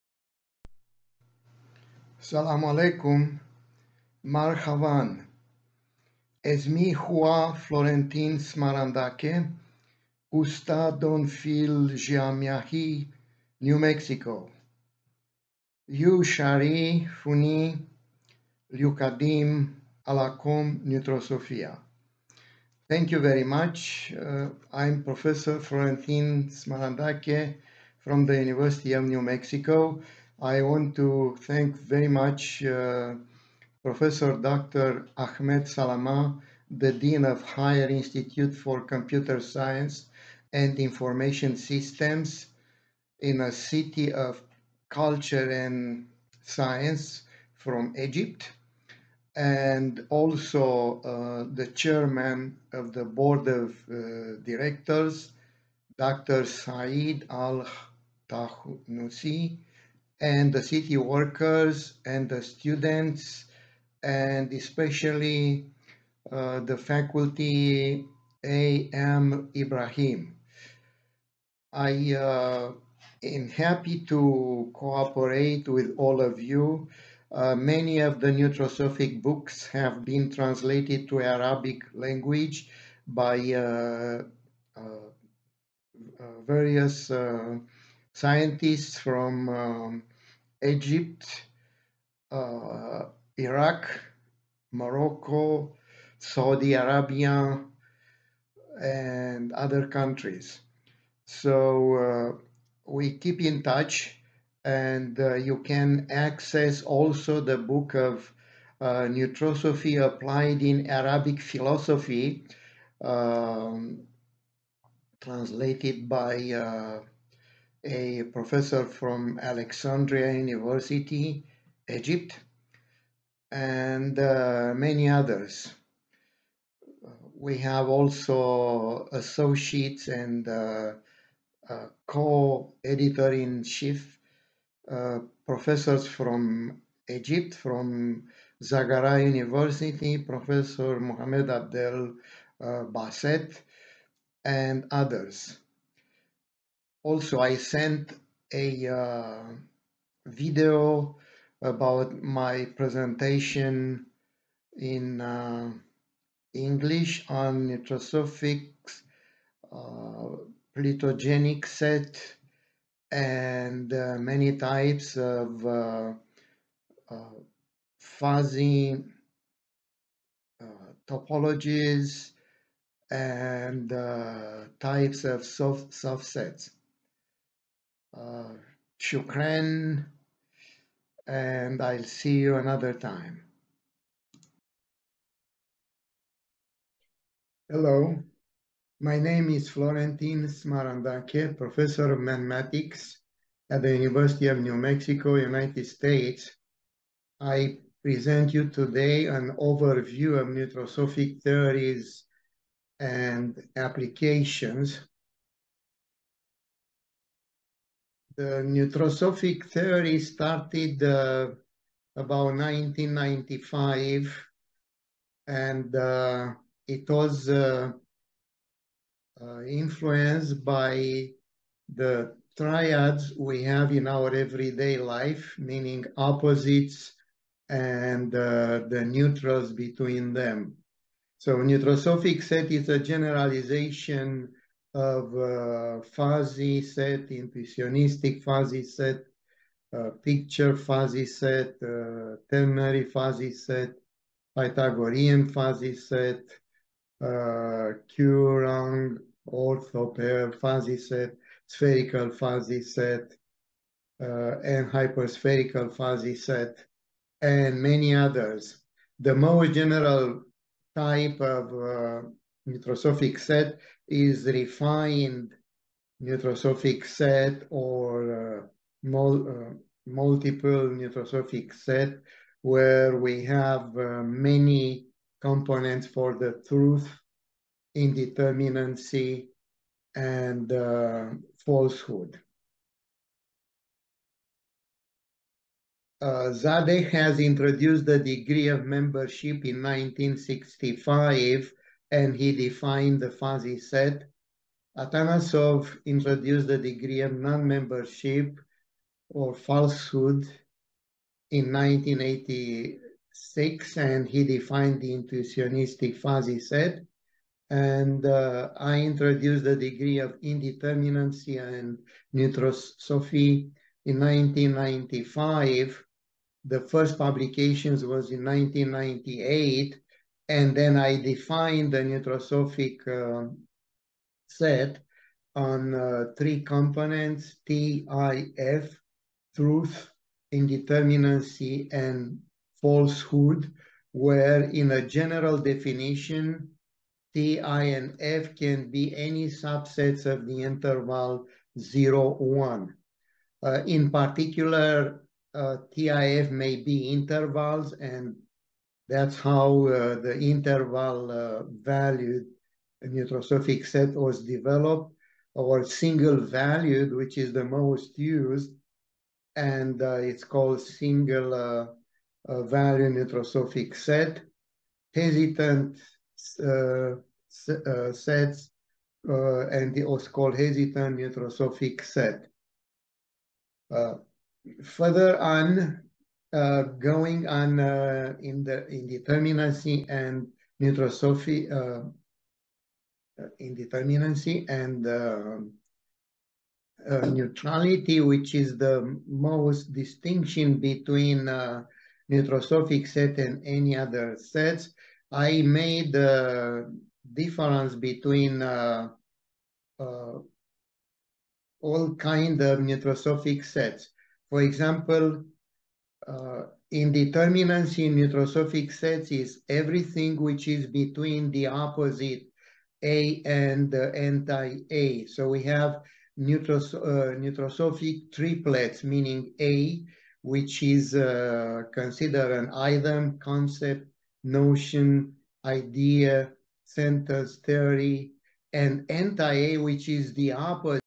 سالة شكر ولقاء مجاضرة مع منظومة العمل بمدينة العلوم والثقافة من العالم الأمريكي فلورنتن سمارانداكة
رسالة شكر ولقاء مع منظومة العمل  بالمعهد العالي لعلوم الحاسب ونظم المعلومات ومدينة العلوم والثقافة بقيادة أ.د. سيد التوتسي  من العالم الأمريكي فلورنتن سمارانداكة عالم الرياضيات وأنظمة النيتروسوفيك ورئيس جامعة نيوميكسيكو بأمريكا ورئيس المجمع العالمى لعلوم النيتروسوفيك ومحاضرة عن النيتروسوفيك وتطبيقاته لطلاب والباحثين